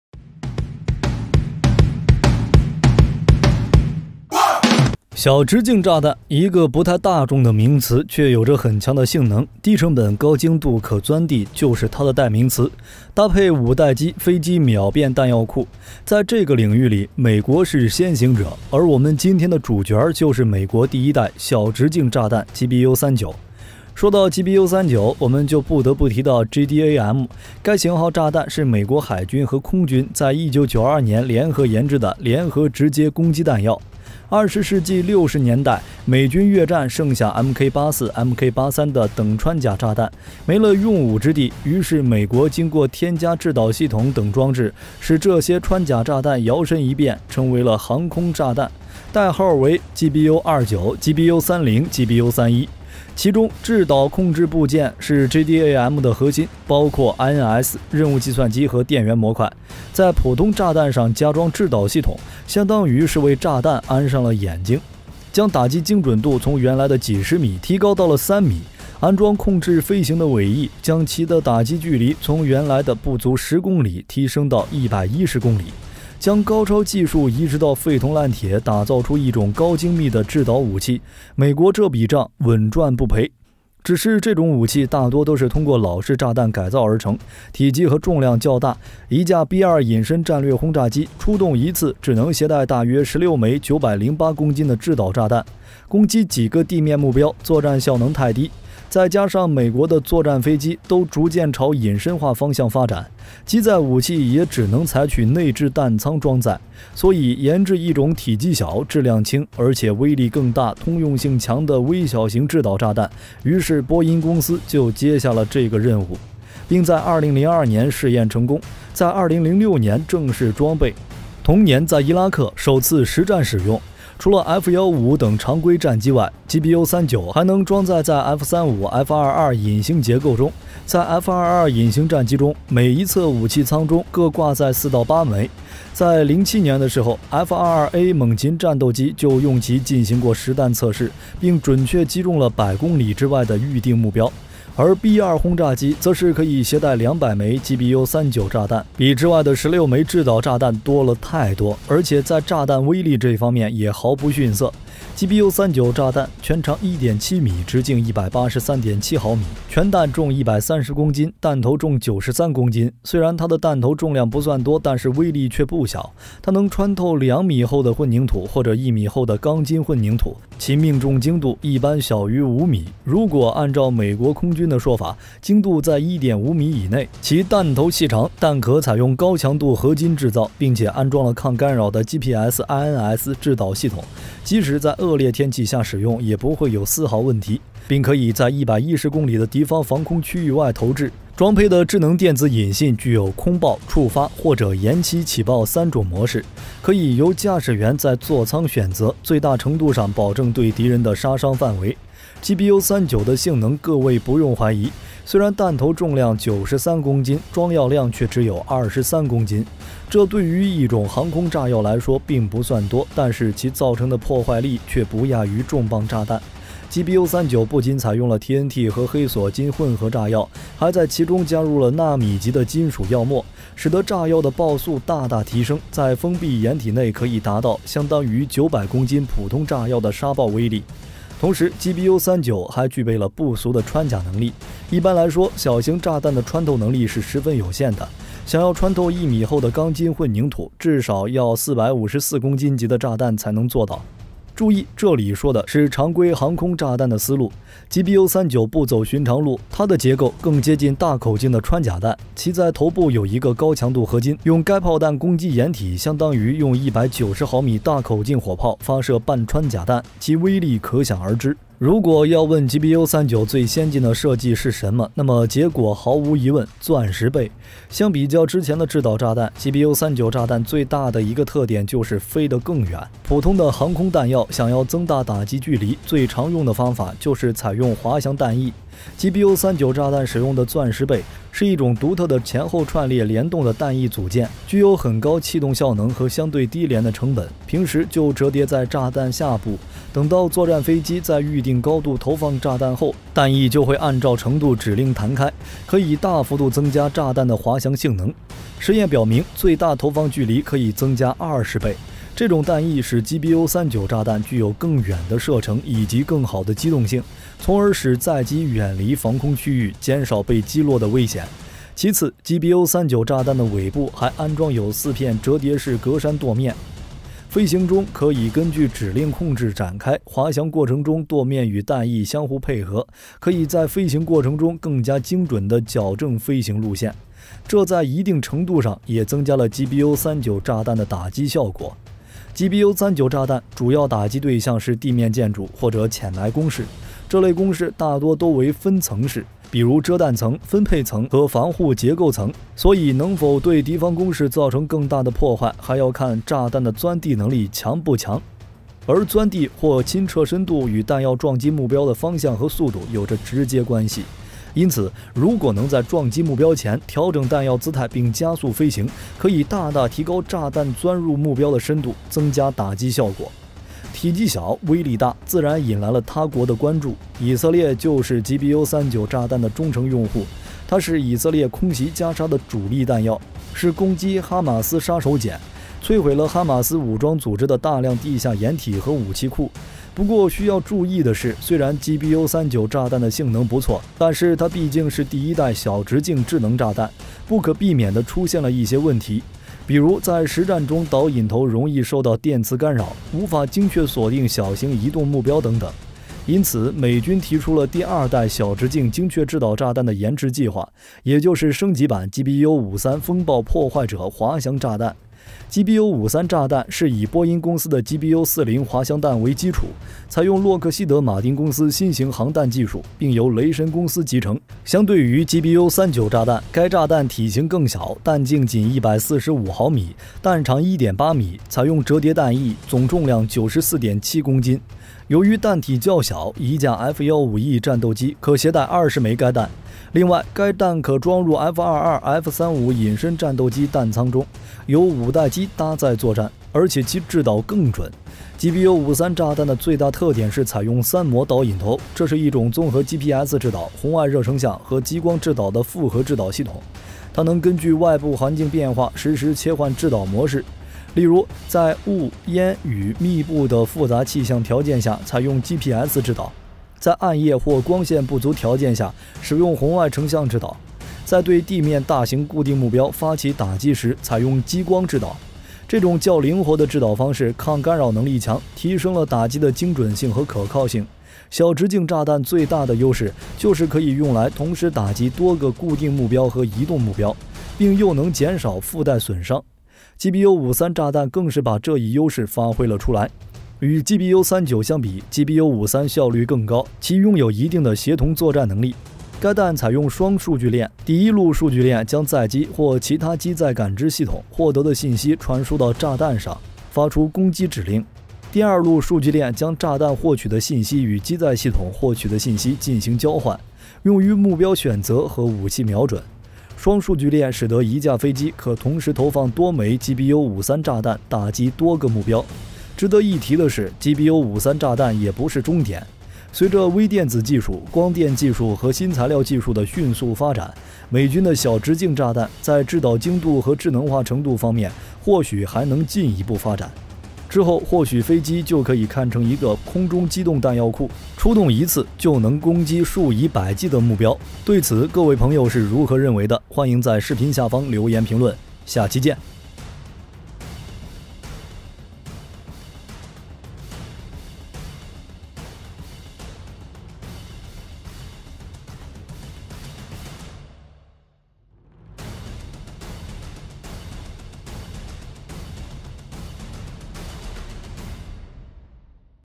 美國GBU-39小直徑炸彈，安裝了抗干擾的GPS/INS制導系統，能穿透2米厚的混凝土，精度在1.5米以內，具有空爆、觸發或者延期起爆三種模式
GBU-39小直徑炸彈（Small Diameter Bomb，SDB）是一種250磅（110公斤）重的導引炸彈，由於這種炸彈體積小和重量輕，所以每架戰機可攜帶更多的炸彈而在每次飛行中較以前攻擊更多目標。大多數美國空軍 戰機可以在原使用BRU-61/A掛架（可掛載1枚MK84 2000磅低阻力通用炸彈 ）處，裝設一組4枚的小直徑炸彈投射器。
The GBU-39 Small Diameter Bomb (SDB) is a 250-pound (110-kilogram) guided bomb that can carry more per fighter due to its small size and light weight. Attacks more targets per flight than before. Most USAF fighters can install a set of 4 Small Diameter Bomb Projectors in place of the original BRU-61/A pylon (which can carry 1 MK84 2000 lb low drag general purpose bomb).
火力就是正義 GBU-39小直徑炸彈